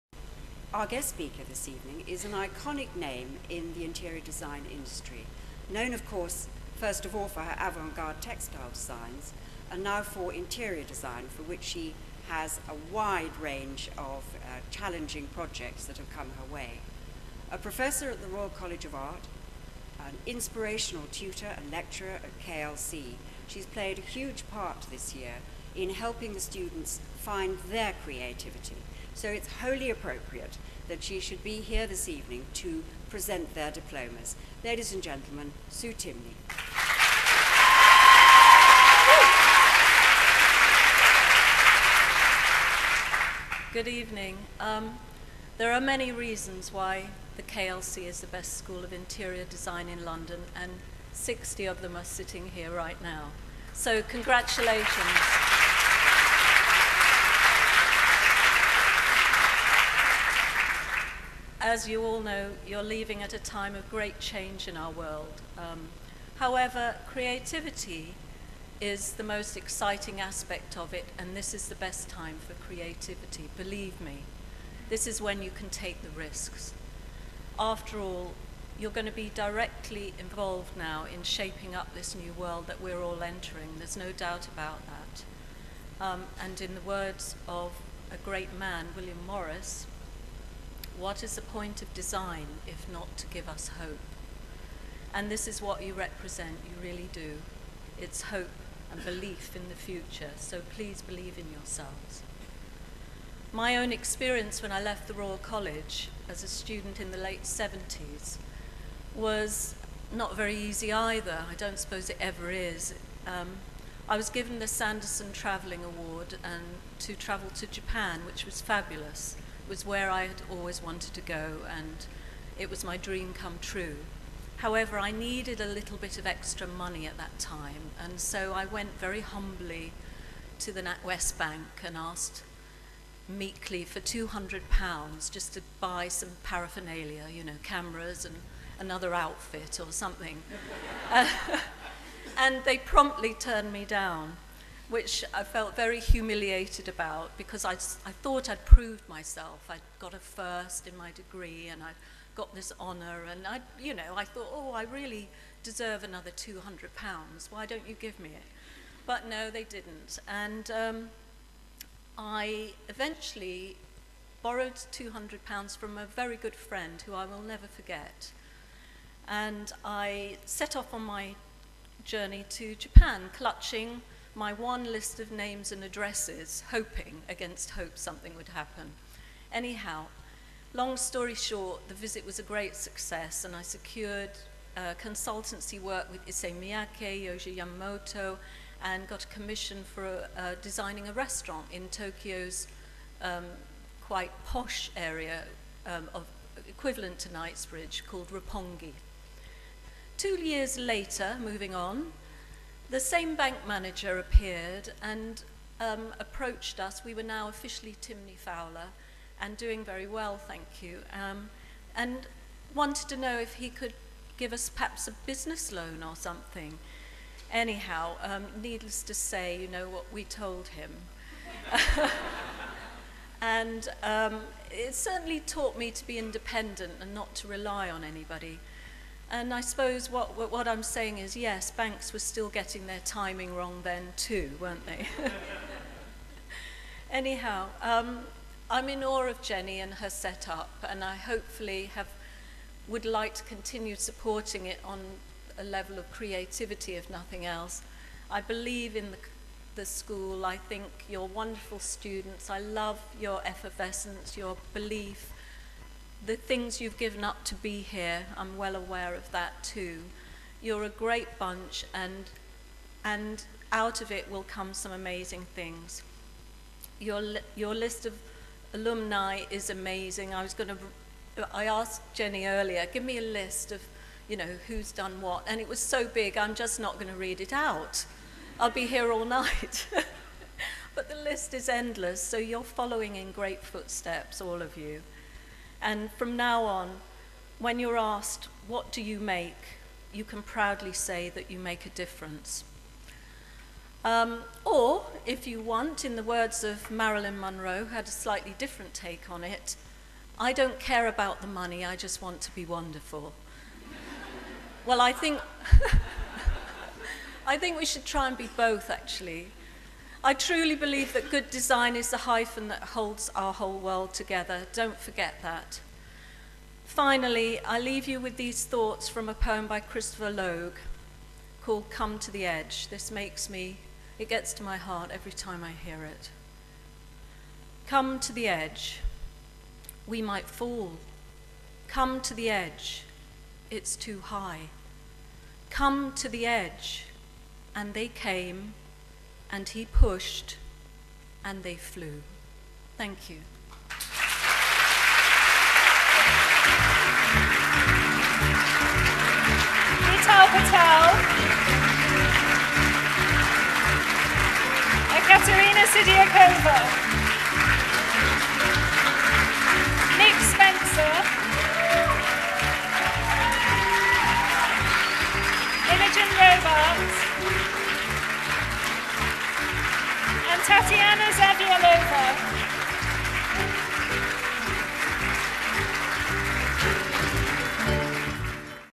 Sue Timney: KLC at RIBA
Guest speaker Sue Timney at the KLC Awards 2009